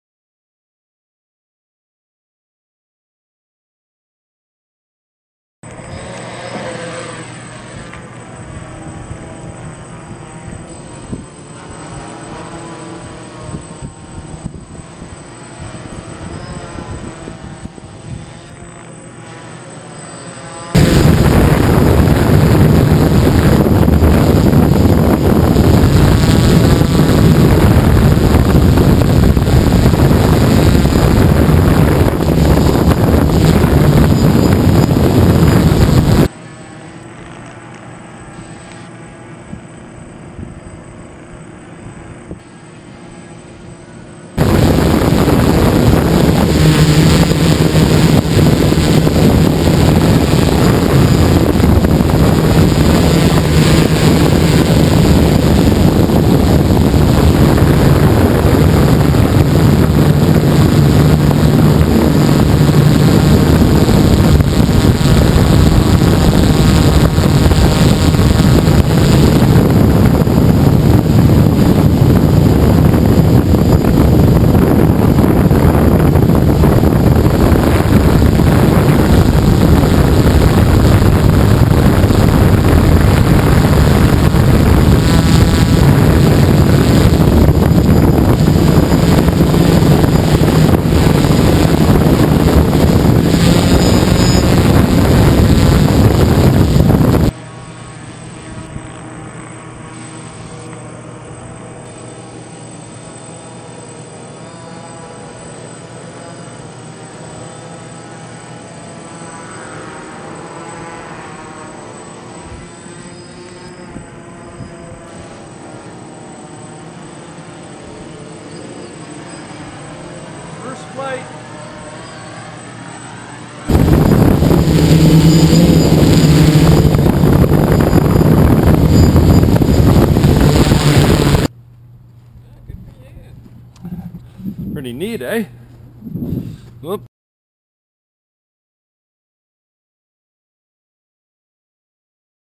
dji550 first flight
My first flight with the DJI 550 Flame wheel & GoPro camera